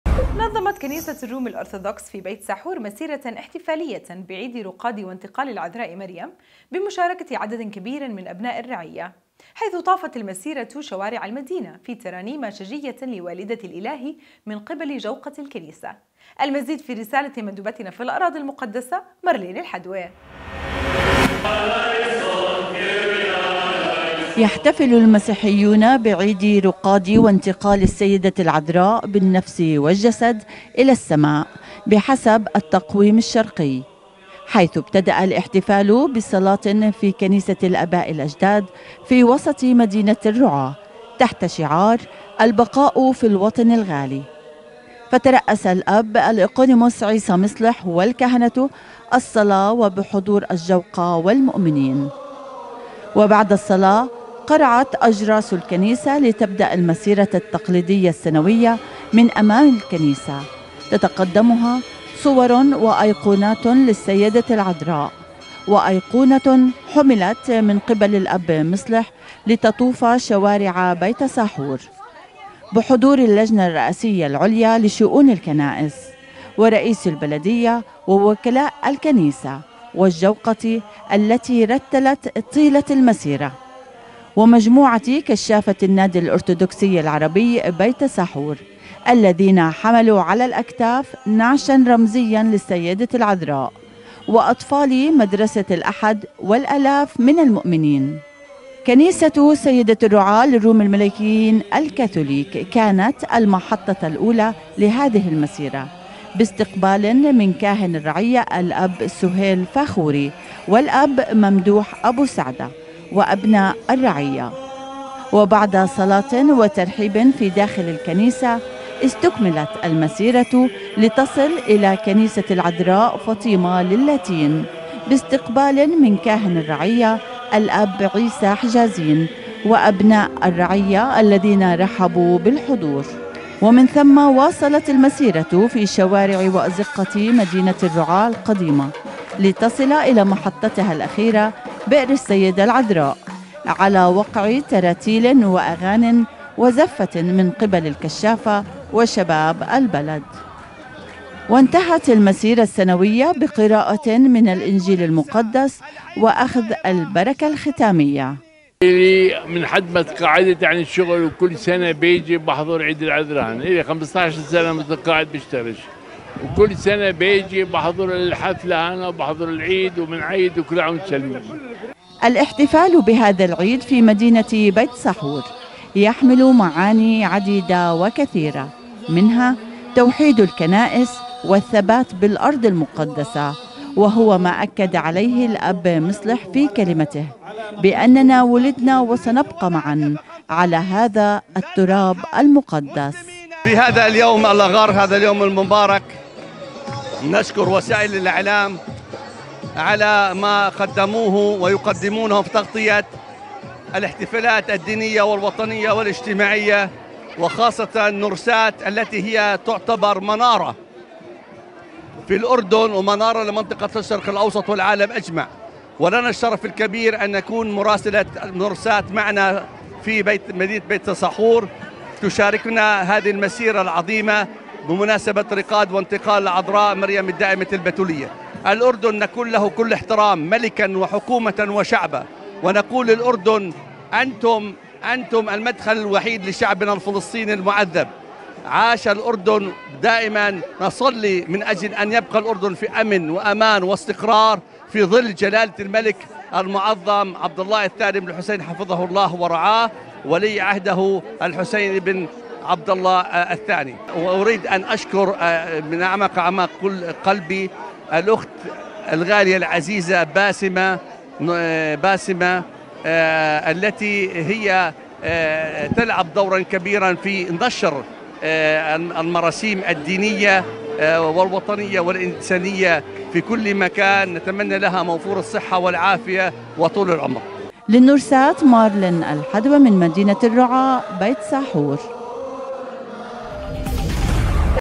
بيت ساحور مسيرةً احتفاليةً بعيدِ رقاد وانتقالِ العذراءِ مريم بمشاركةِ عددٍ كبيرٍ من أبناءِ الرعيه
نظمت كنيسةُ الرومِ الأرثوذكس في بيت ساحور مسيرةً احتفاليةً بعيدِ رقاد وانتقالِ العذراءِ مريم بمشاركةِ عددٍ كبيرٍ من أبناءِ الرعيه / حيث طافت المسيرةُ شوارعَ المدينه في ترانيمَ شجيةٍ لوالدةِ الإلهِ من قِبل جوقةِ الكنيسه / المزيد في رسالة مندوبتنا في الأراضي المقدسة مارلين الحدوه